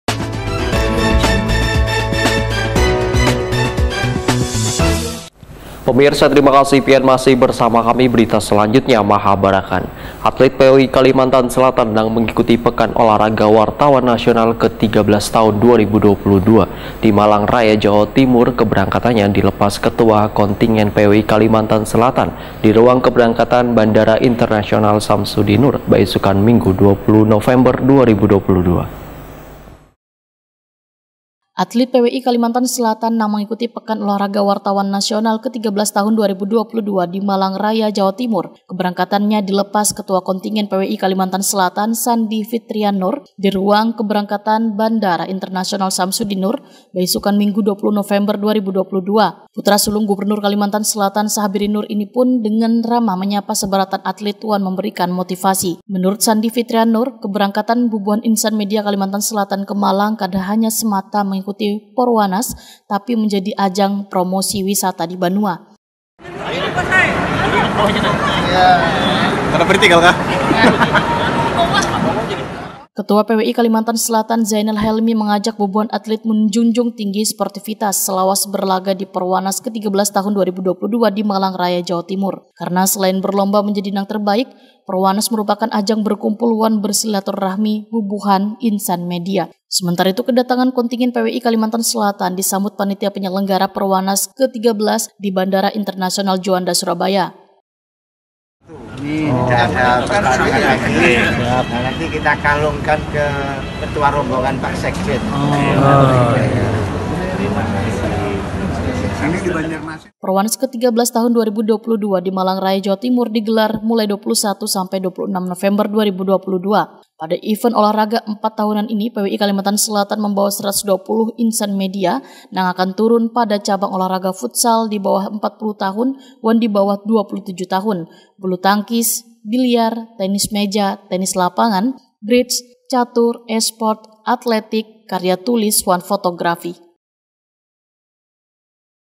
PWI KALIMANTAN SELATAN MEMBAWA 120 INSAN MEDIA
Atlet PWI Kalimantan Selatan nang  mengikuti Pekan Olahraga Wartawan Nasional (Porwanas) XIII tahun 2022 di Malang Raya Jawa Timur keberangkatannya dilepas Ketua Kontingen PWI Kalimantan Selatan - Sandi Fitrian Noor di ruang keberangkatan Bandara Internasional Syamsudin Noor/ baisukan Minggu 20 November 2022//